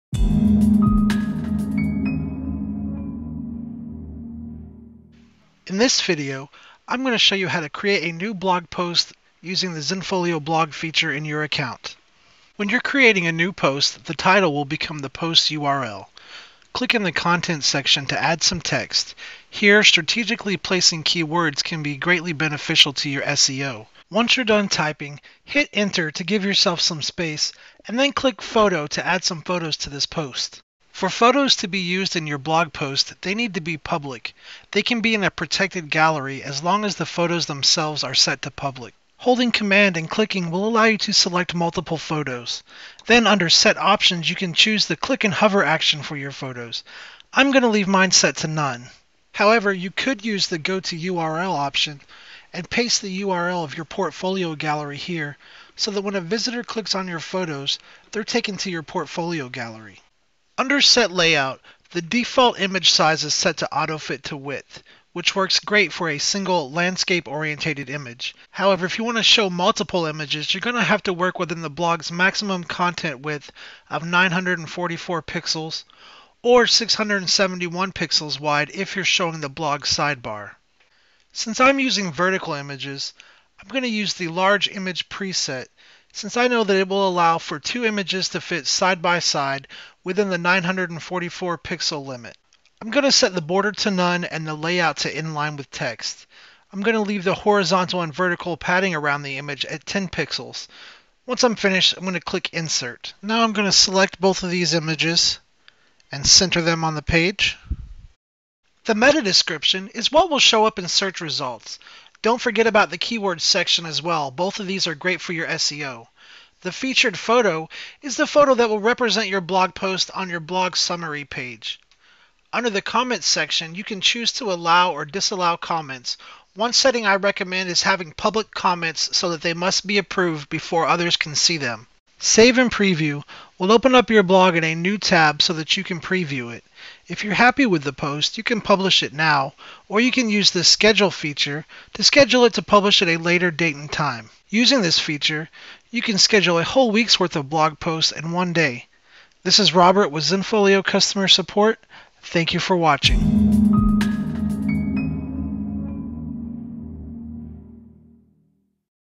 How to blog - creating a new blog post in Zenfolio | Zenfolio Classic
Blogging is one of the best ways to get traffic to your site, and enhance your SEO. See how to make a post and add photos.